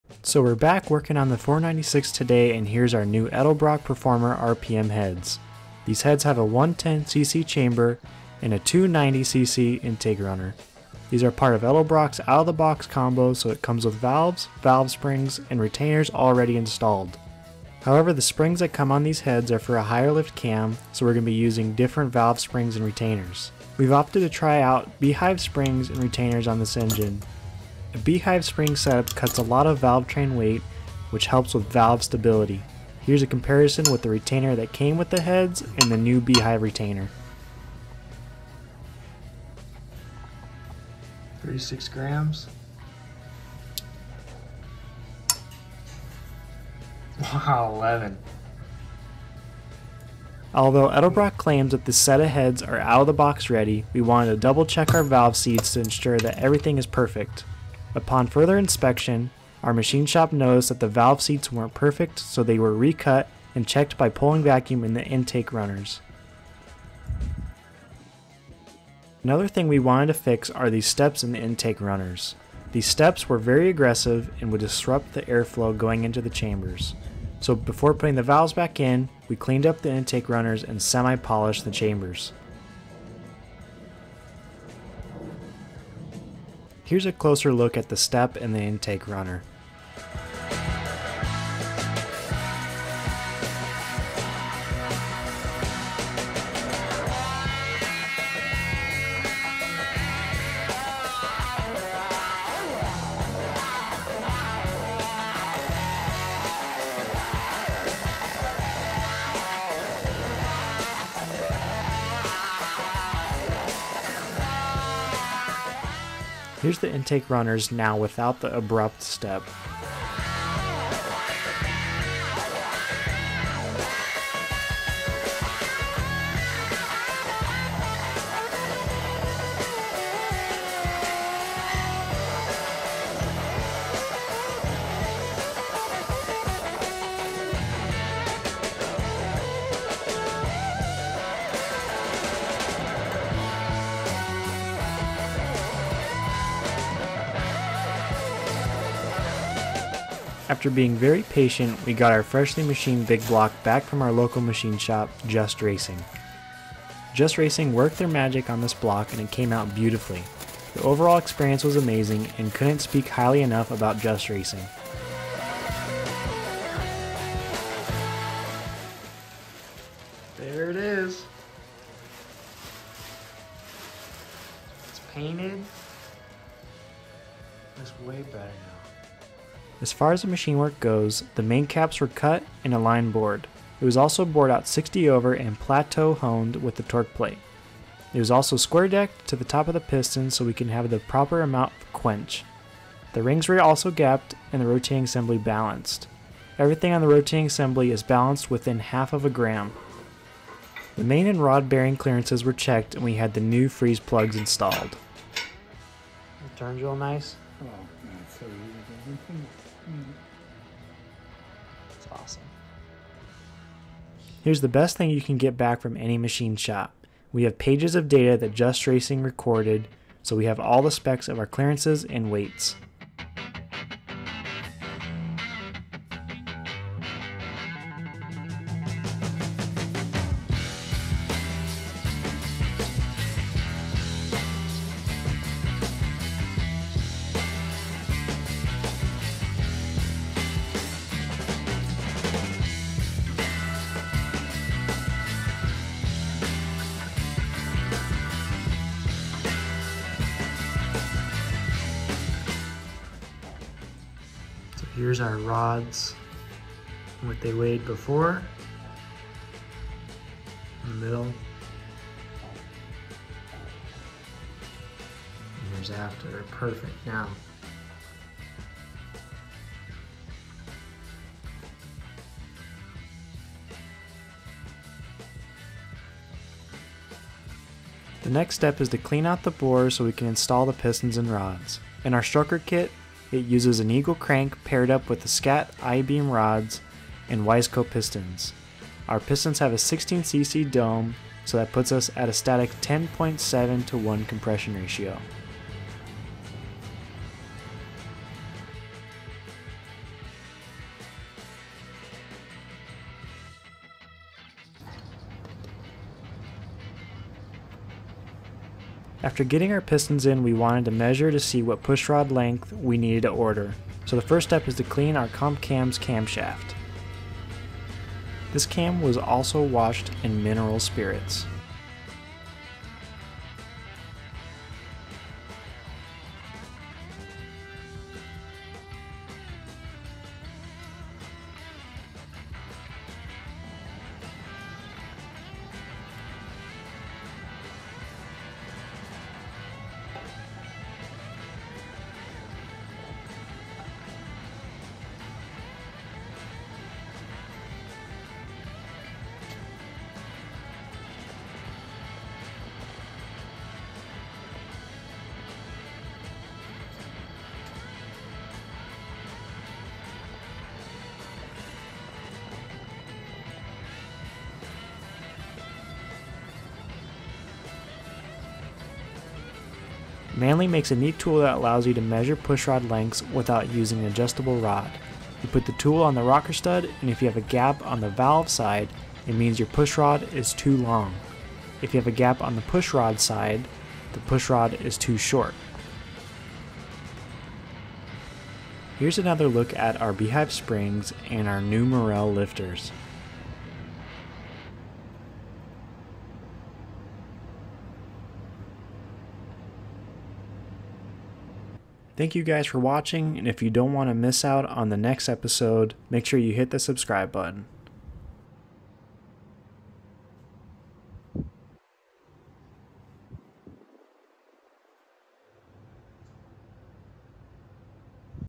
496 Big Block Chevy: Final Assembly!
The 496 Big block is back from our local machine shop and its ready for final assembly! We go through some engine specs while installing the rotating assembly and measuring for pushrods.